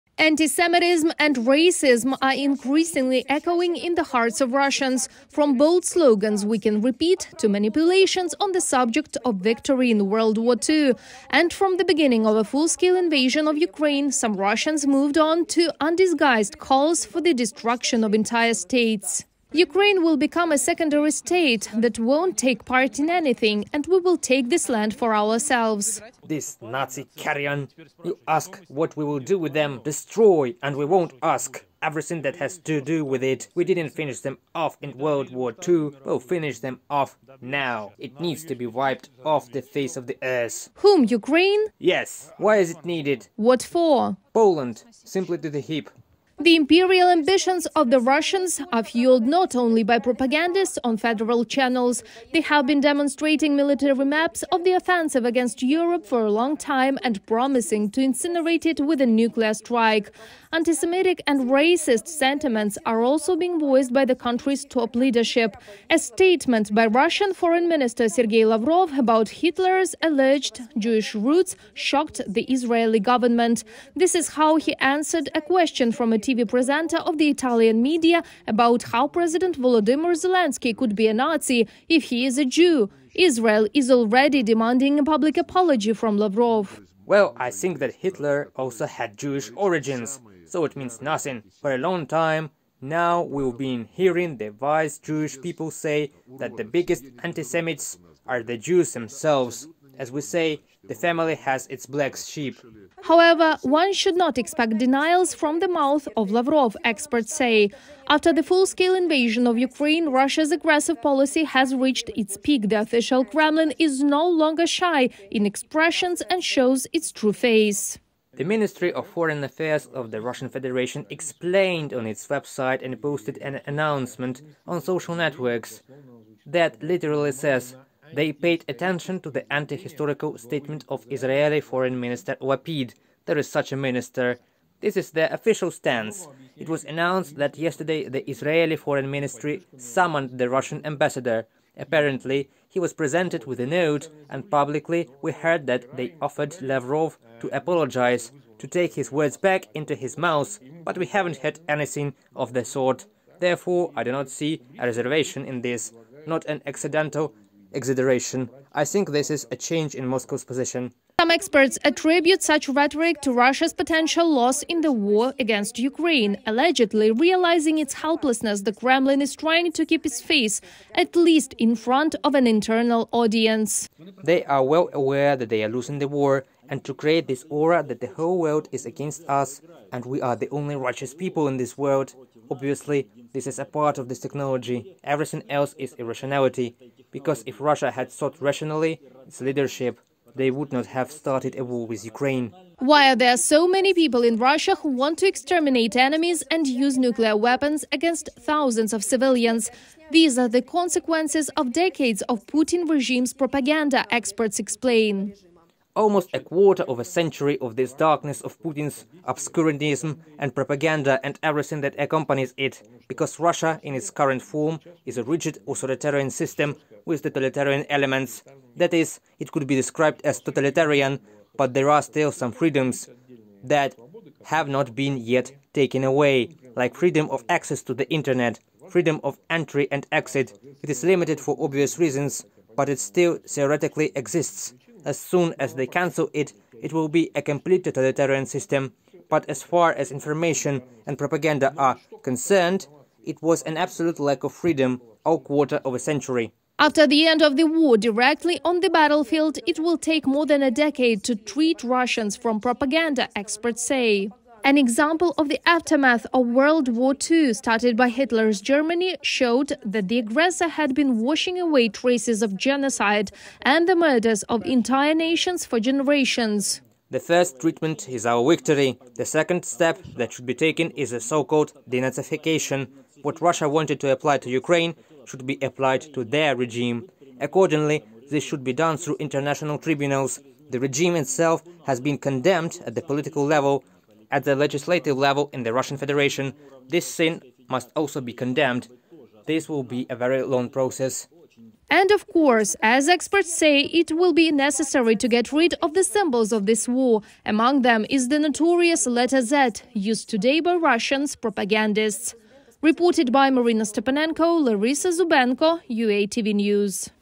Domestic anti-Semitism and racism in Russia
Russian Foreign Minister Sergey Lavrov recently said that Hitler had Jewish roots and that Jews have always been the main anti-Semites. Israel is outraged by this position and awaits an official apology. Volodymyr Zelenskyy also reacted to the anti-Semitic position of the Russia's chief diplomat. The President of Ukraine is shocked that such a statement was made on the eve of the anniversary of the victory over fascism. However, this is not the first time that Russia has demonstrated its imperial and racist ambitions at the international level.